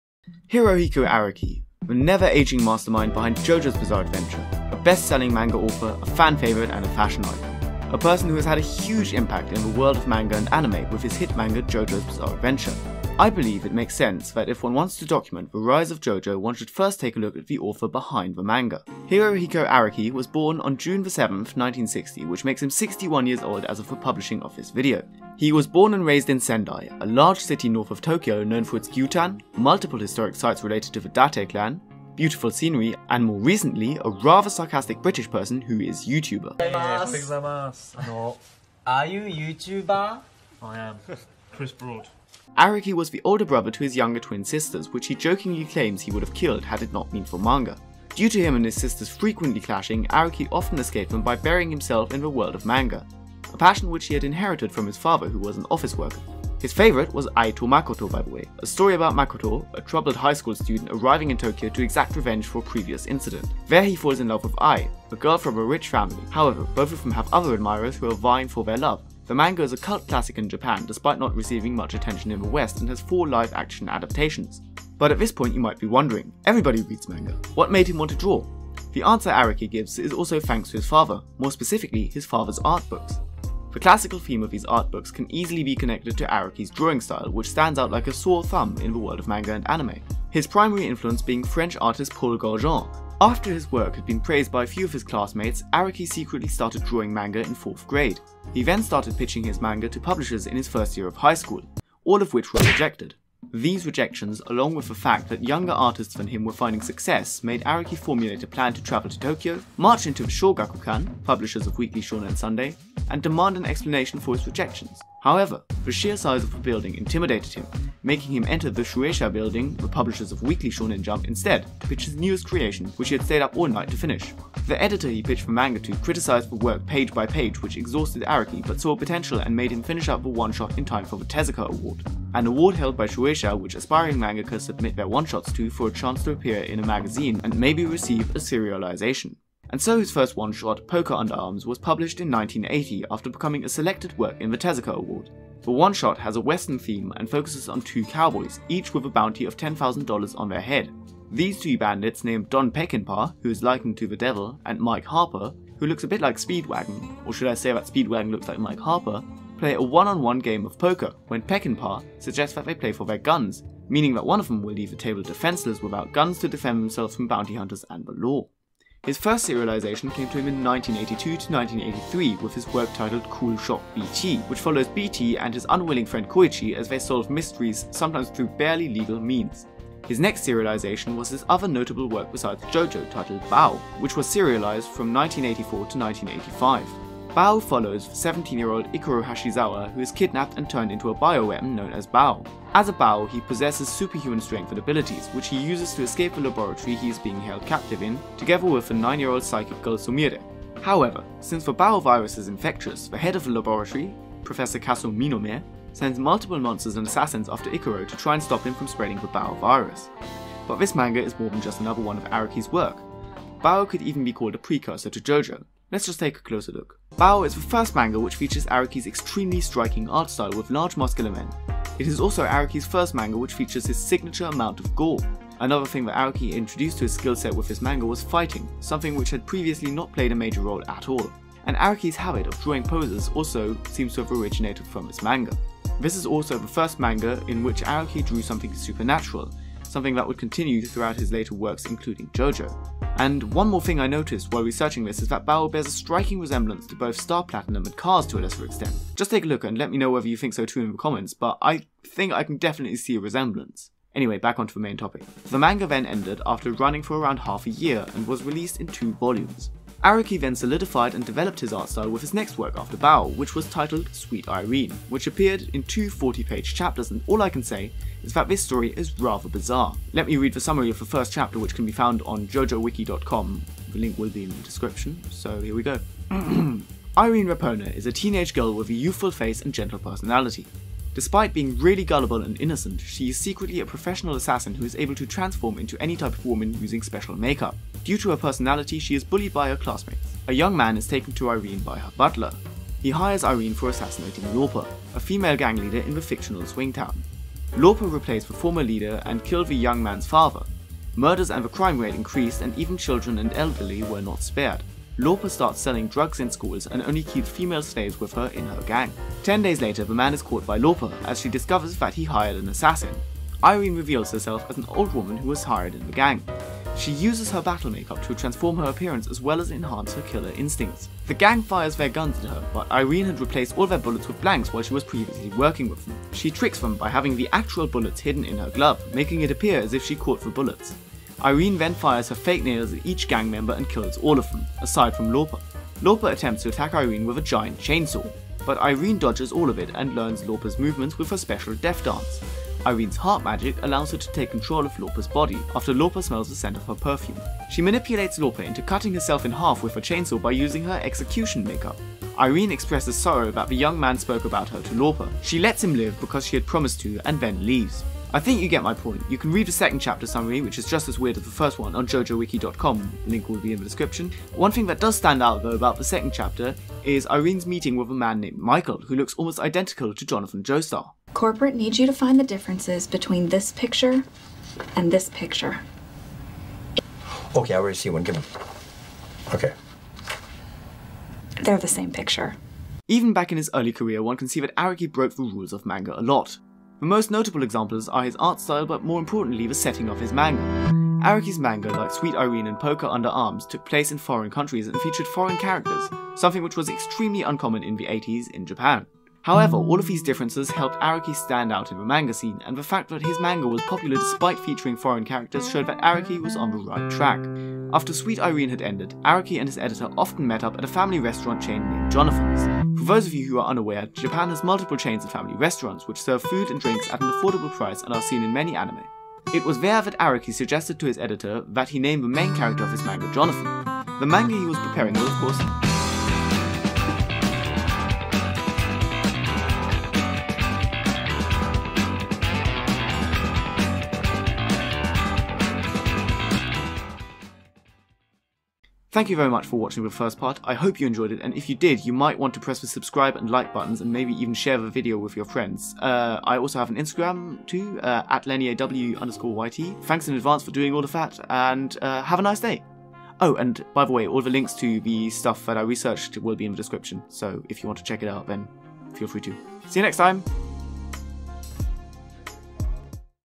Hirohiko Araki before JoJo's Bizarre Adventure
This video is my biggest project yet and took ages to make so I hope you enjoy. I don't quite know when part 2 and 3 will be released but I want them all to be released in 2021. With stone ocean out I hope I can help you look back.

Please let this video get me to trash taste so I can hang with the boys aka Cdawgva theanimeman and gigguk